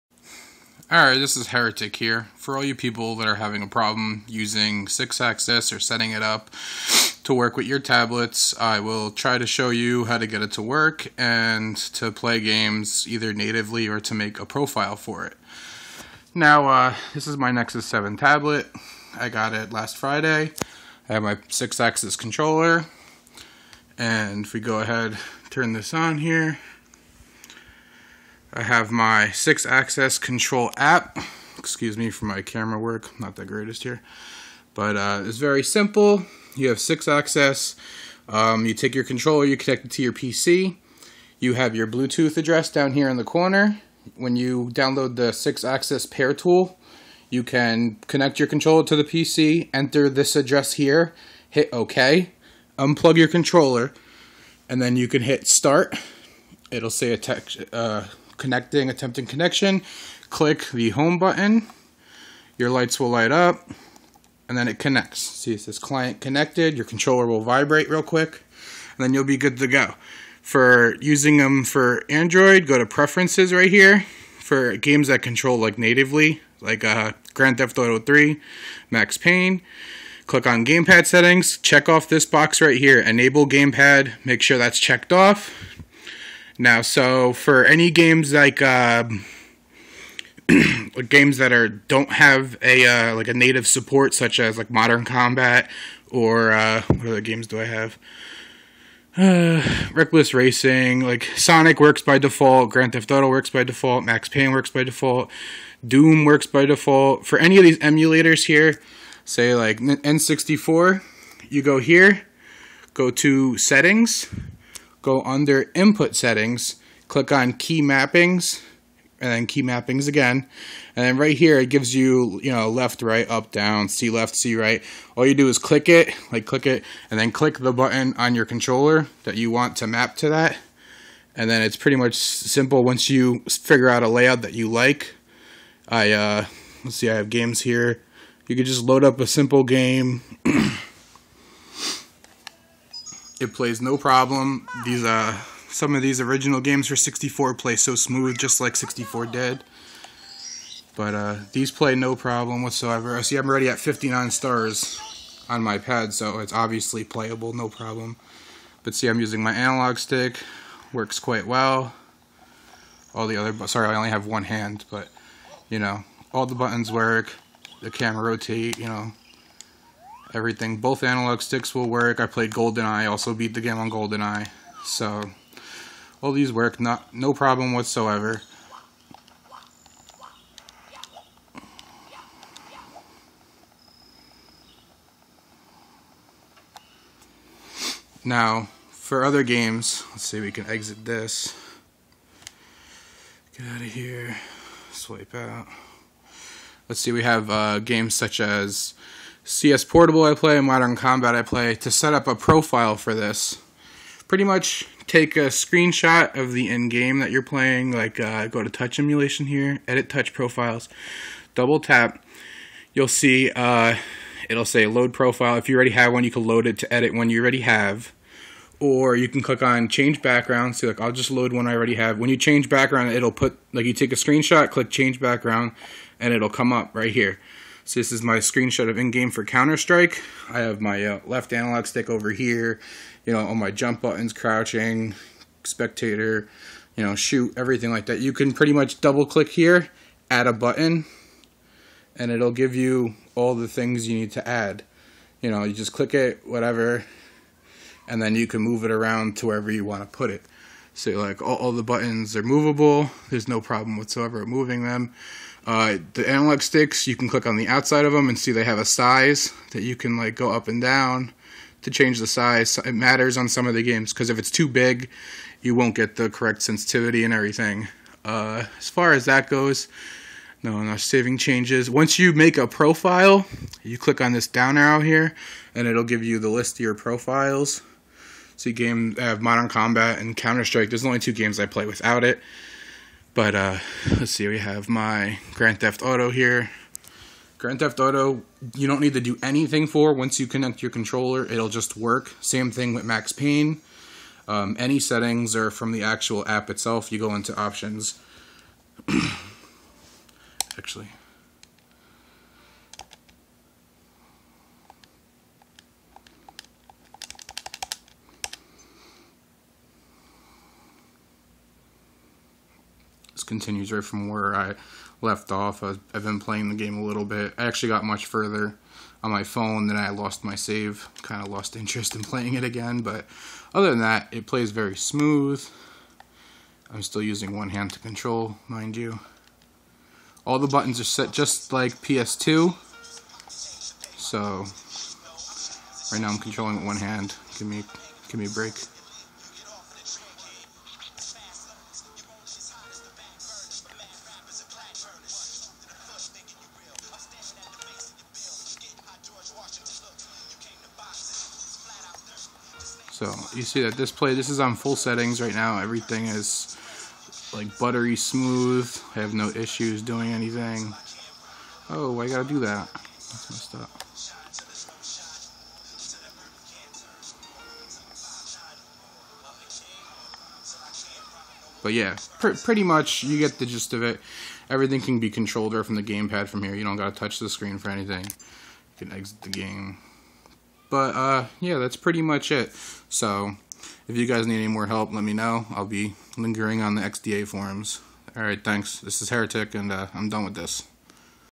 Sixaxis controller setup nexus 7 android
(root is required)  How to set up and play android games using six axis ps3 controller. For profiles and support for nexus 7.

Get Six axis pai